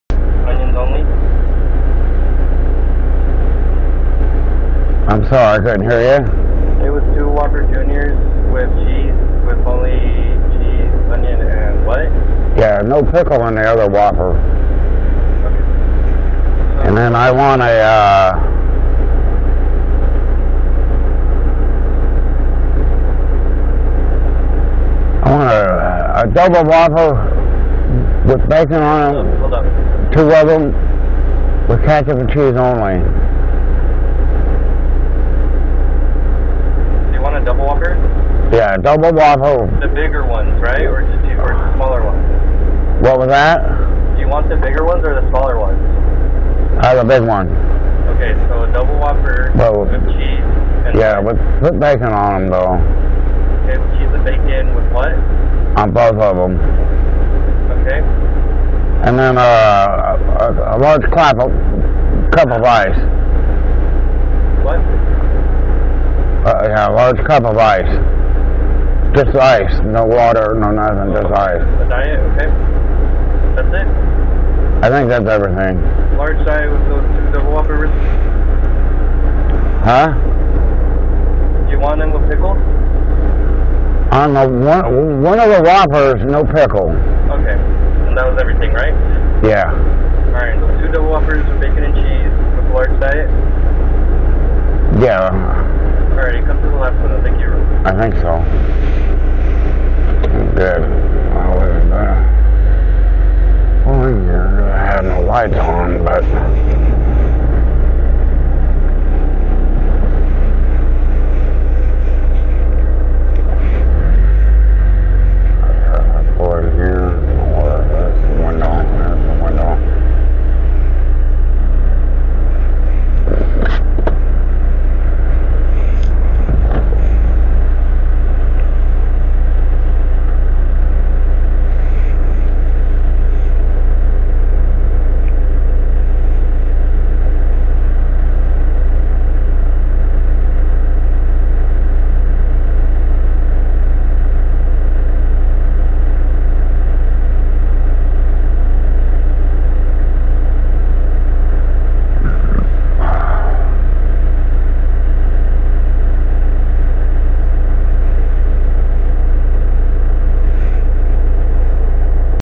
criminals break in through static plus the rattling talks too
I hear them say something about black mail. idiots using rattling sounds to try to brainwash me or form words off their stupidity